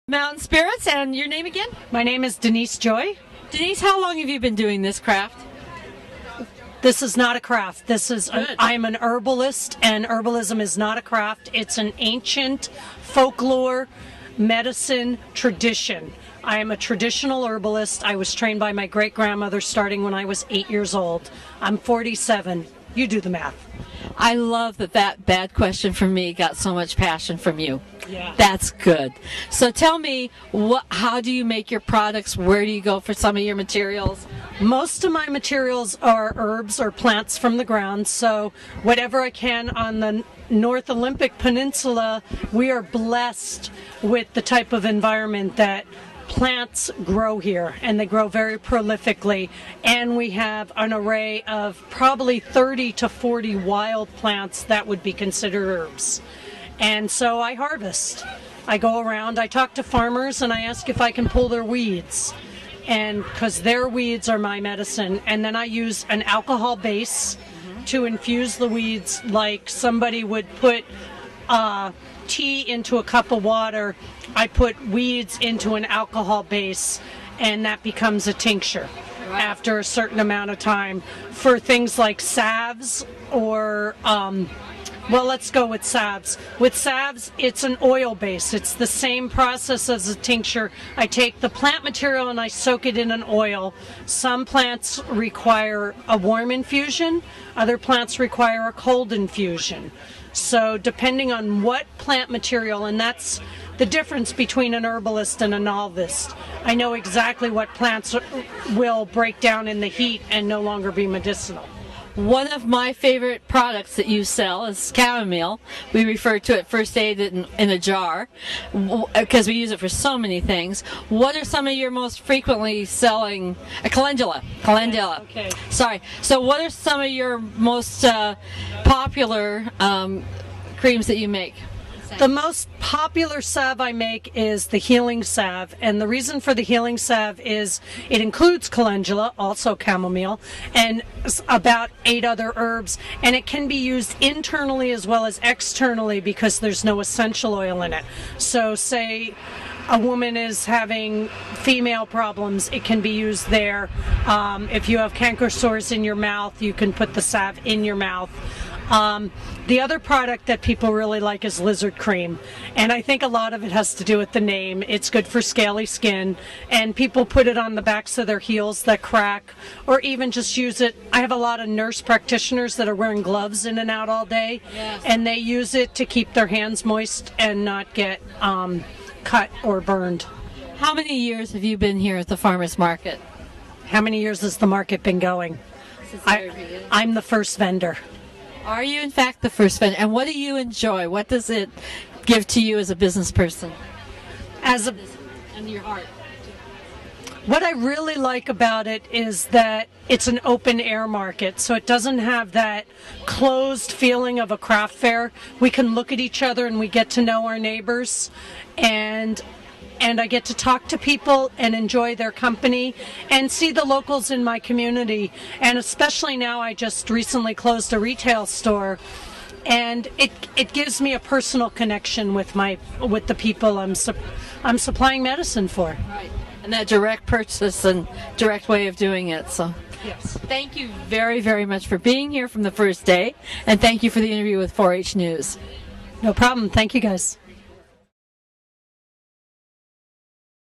Denise Joy - Port Townsend Herbalist
Denise Joy, herbalist at the Port Townsend Farmer's Market, talks with Katherine Baril, WSU Jefferson County Extension director, about her herbal medicines.